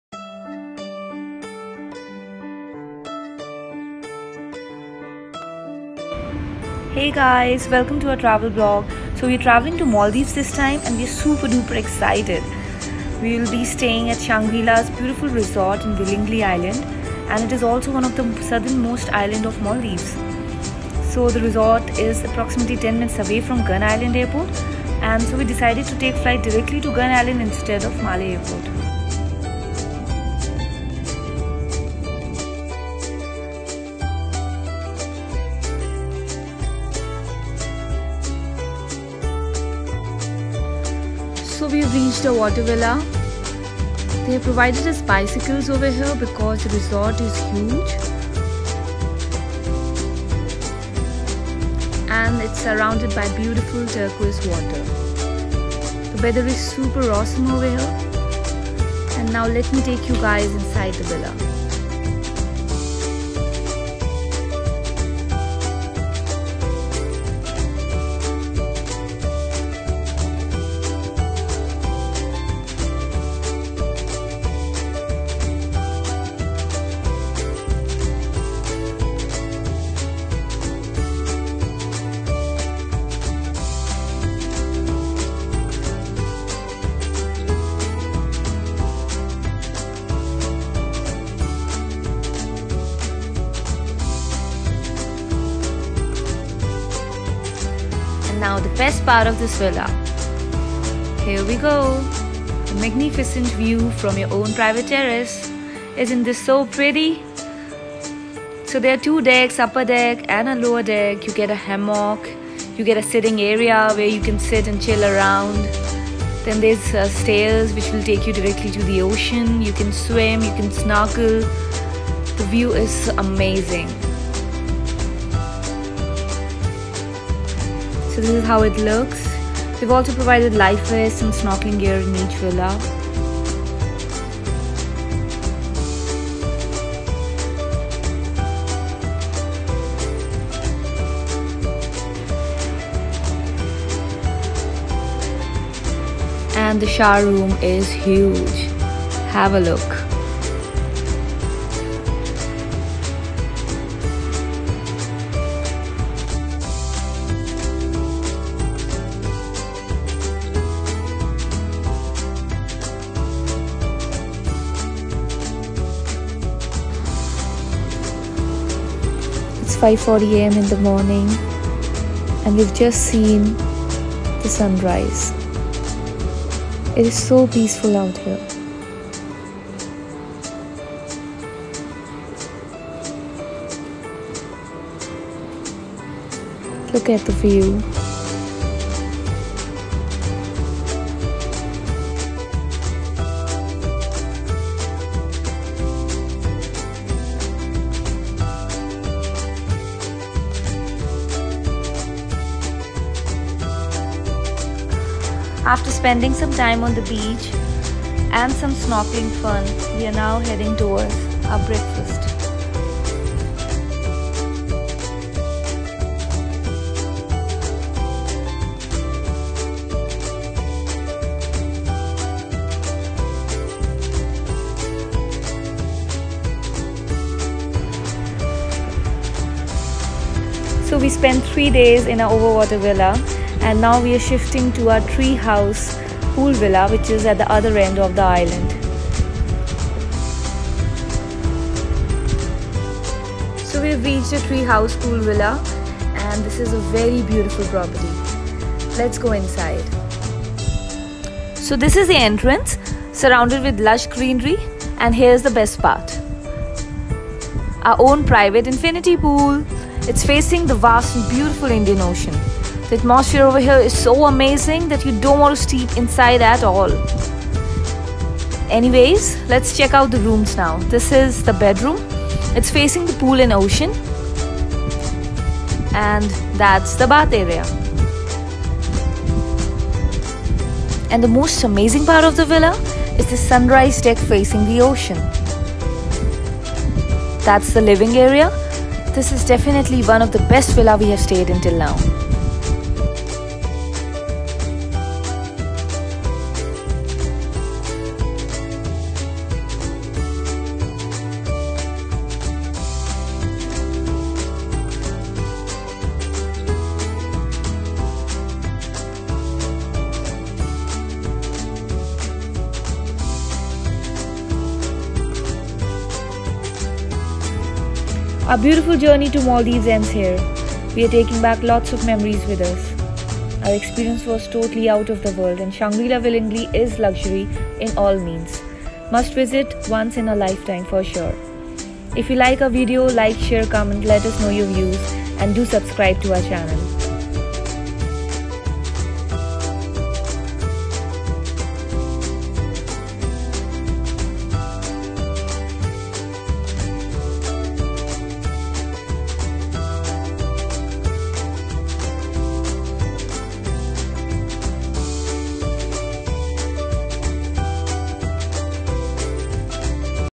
Best Luxury Resort in Maldives - The Shangri-La Villingili Resort
This trip was designed to show what a relaxing luxury holiday in the Maldives looks like. We stayed for 6 days in Shangri-la Villingili Resort.

P.S. Sorry for the shaky video.

We will be sharing more on our never ending adventures! :)